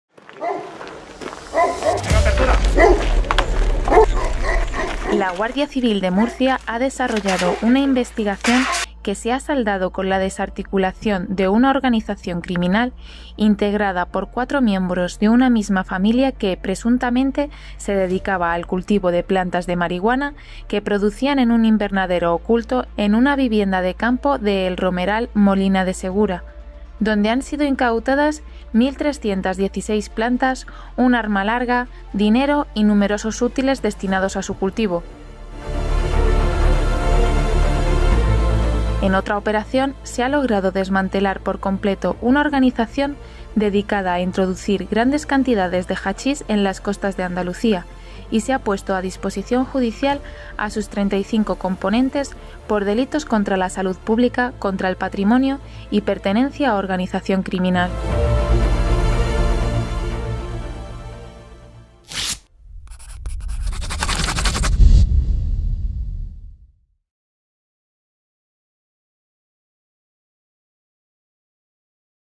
Operaciones Comienzo y Antmarmo. Desarticuladas organizaciones criminales de tráfico de drogas
La Guardia Civil desarticula una organización que introducía grandes cantidades de hachís en Andalucía e incauta de más de 2.000 plantas de marihuana en Murcia

La organización que introducía el hachís en las costas de Andalucía, utilizaba el método conocido como FONDEO y se le ha incautado más de una tonelada de hachís
En Murcia, los cuatro miembros de un clan familiar han sido detenidos, como presuntos autores de los delitos de cultivo, elaboración y tráfico de droga, defraudación de fluido eléctrico y de pertenencia a organización criminal
La Guardia Civil,  en el marco de la Operación Comienzo ha desmantelado una organización criminal dedicada a introducir grandes cantidades de hachís en las costas de Andalucía poniendo a disposición de la Autoridad Judicial a sus 35 componentes. En otra operación en Murcia, los cuatro miembros de un clan familiar han sido detenidos como presuntos autores de los delitos de cultivo, elaboración y tráfico de droga, defraudación de fluido eléctrico y de pertenencia a organización criminal. Se han incautado más de 2.000 plantas de cannabis sativa, cerca de dos kilos cogollos de marihuana, un arma larga y dinero, en dos chalets con conexión ilegal a la red eléctrica.

Operación Antmarmo

La Guardia Civil de Murcia ha desarrollado una investigación que se ha saldado con la desarticulación de una organización criminal, integrada por cuatro miembros de una misma familia que, presuntamente, se dedicaba al cultivo de plantas de cannabis sativa que producían en un invernadero oculto en una vivienda de campo de El Romeral-Molina de Segura, donde han sido incautadas 1.316 plantas de cannabis sativa, un arma larga, dinero y numerosos útiles destinados a su cultivo. En una segunda vivienda, ubicada a escasos metros de la primera, se ha desmantelado otro invernadero indoor con 702 plantas de cannabis sativa, donde también se han aprehendido cerca de dos kilos de cogollos de marihuana listos para su distribución. Esta segunda fase de la investigación continúa abierta a la espera de identificar a las personas relacionadas con esta plantación.